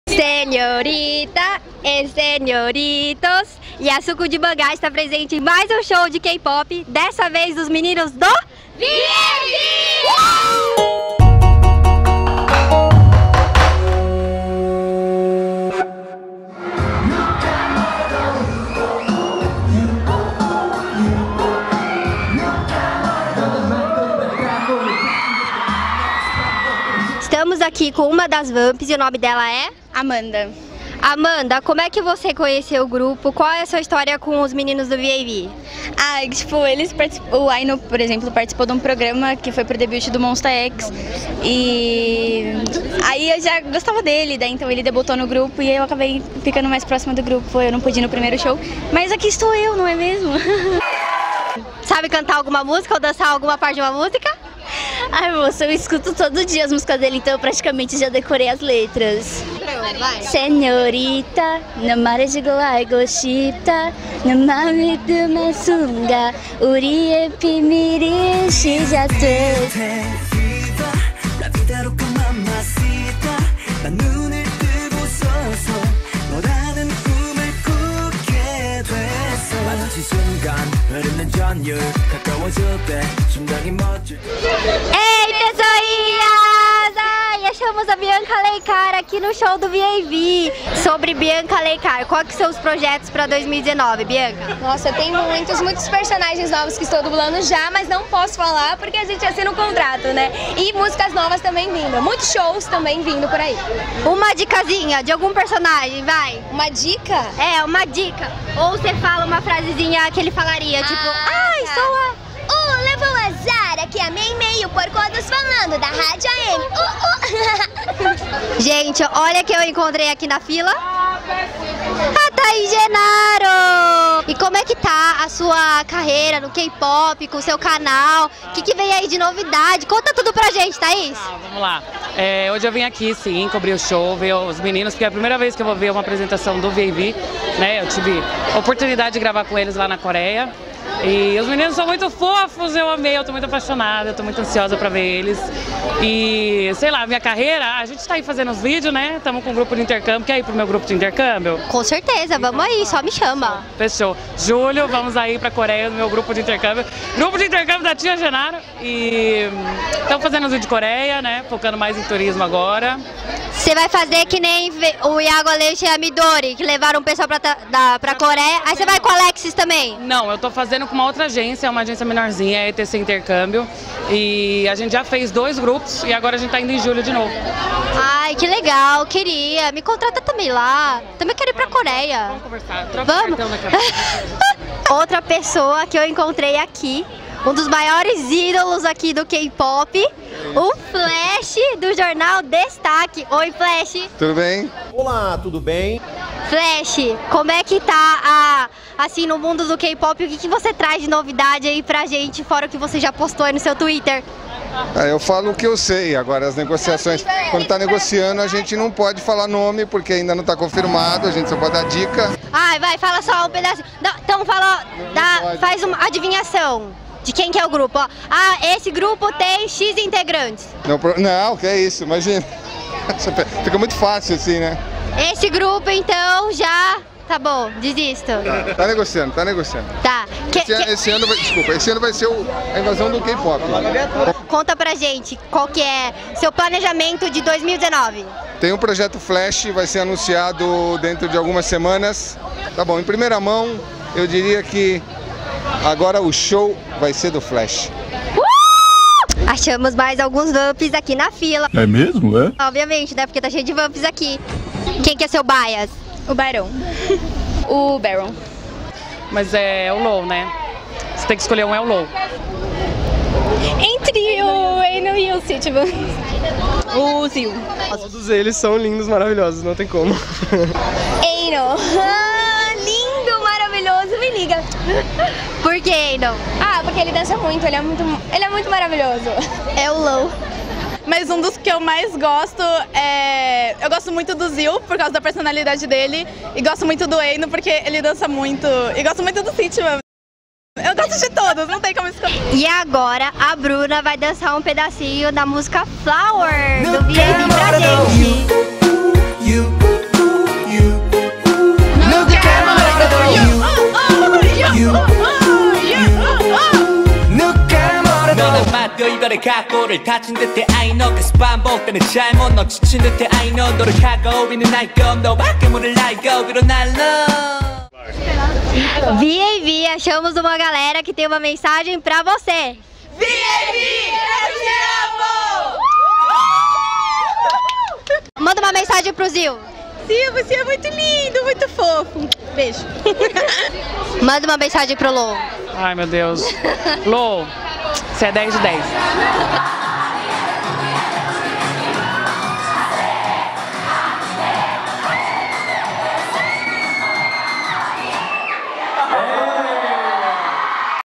VAV EM SÃO PAULO
No dia 24 de fevereiro, estivemos em mais um show K-POP, e dessa vez com o boygroup VAV. Por lá entrevistamos o pessoal da fila e também os influencers: José Norberto Flesch (@jnflesch), Bianca Alencar (@biancaalencaroficial) e Thais Genaro (@thaisgenaroo), que contaram pra gente suas experiências.

EXPERIMENTE O SUCO DE MANGÁ EM OUTRAS MÍDIAS!